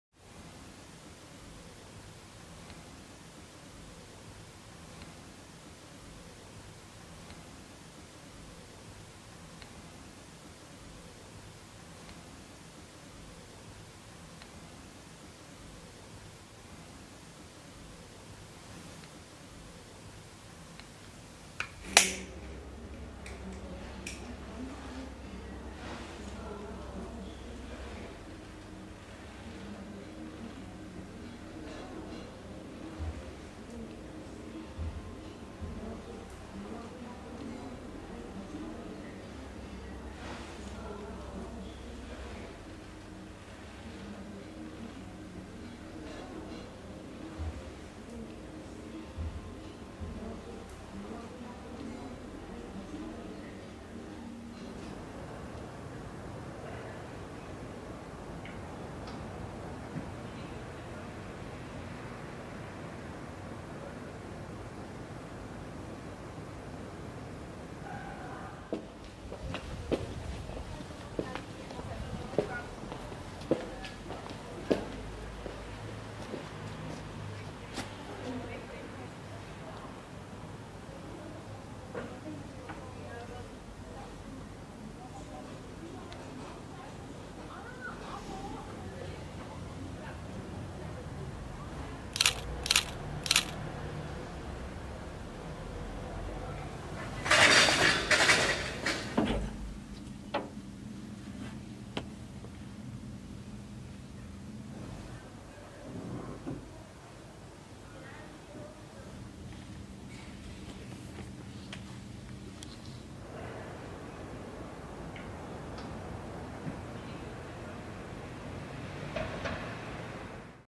Stalking
Kijken en bekeken worden. Een filmpje van Lukas Gevaert.